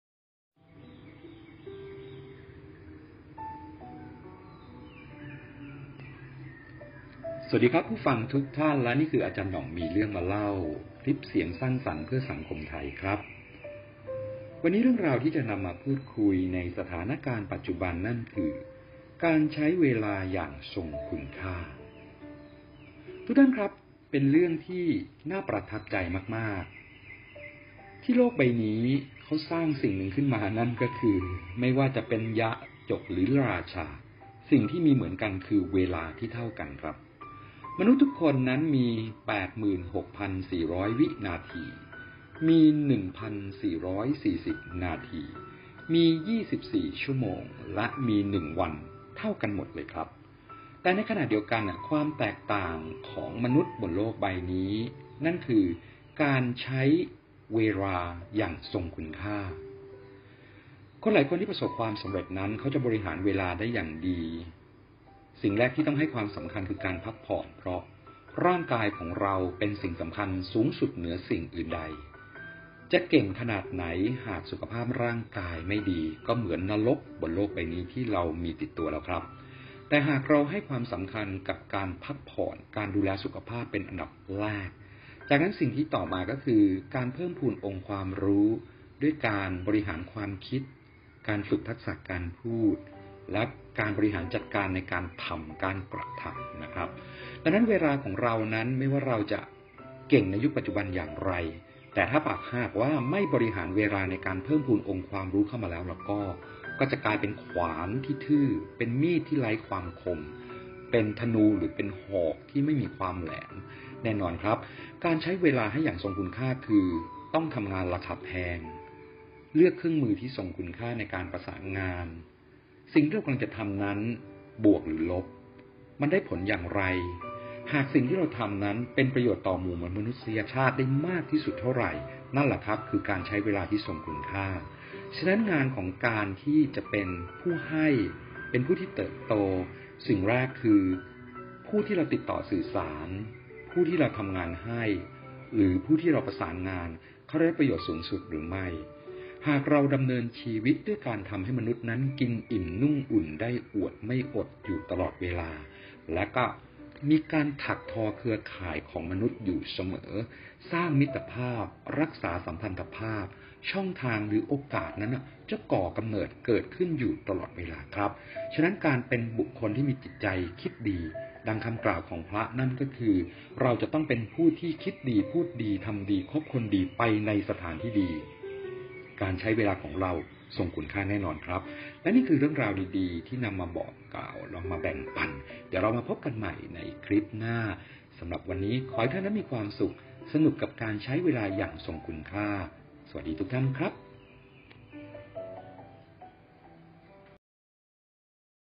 การใช้เวลาอย่างทรงคุณค่า
วันพฤหัสบดีที่ 15 กค.64
คลิปเสียง

โดย อ.ณัฐนัย  โล่ห์สุวรรณ
ประธานผู้ก่อตั้งสถาบันฯWLDC
วิทยากรด้านการสื่อสารและพัฒนาบุคลิกภาพ